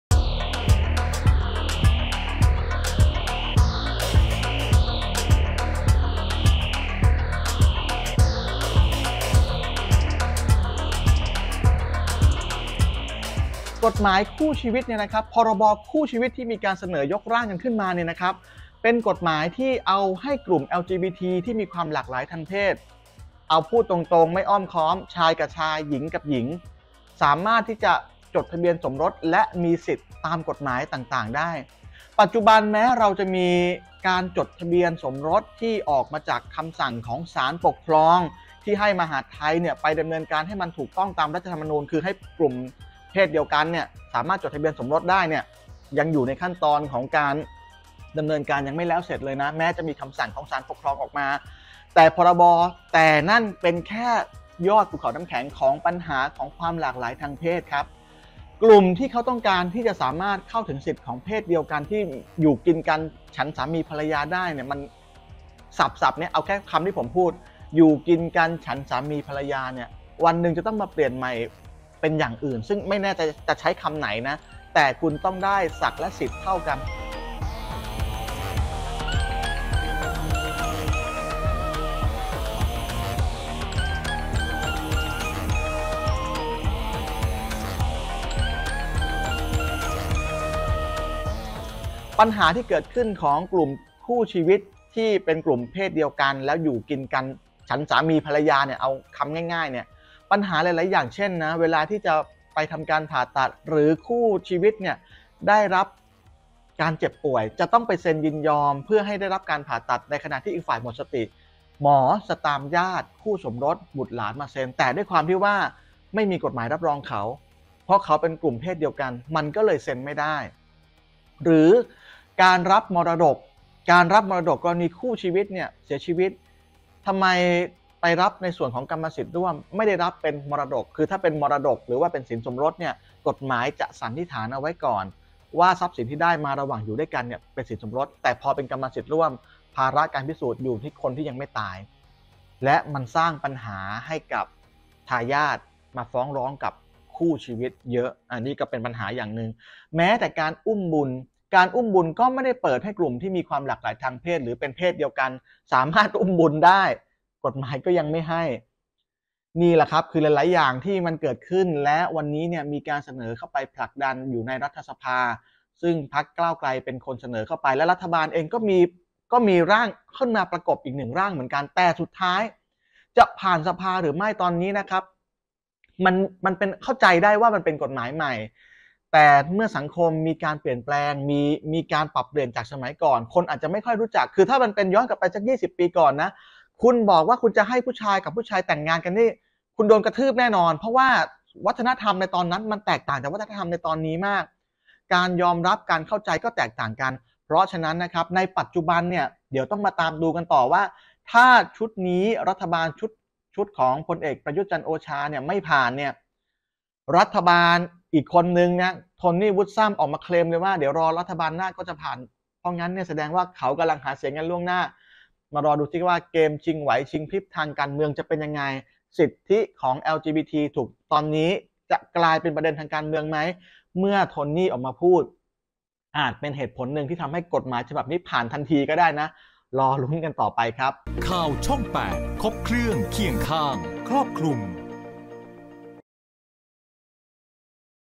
"นักกฎหมาย" สะท้อน "พรบ.คู่ชีวิต" | ข่าวช่อง 8 | 8 มิ.ย. 65
ร่างพรบ.คู่ชีวิต ถูกพูดถึงอย่างมาก ทั้งเห็นด้วย และเห็นต่าง นักกฎหมายดัง เปิดมุมมองประเด็นนี้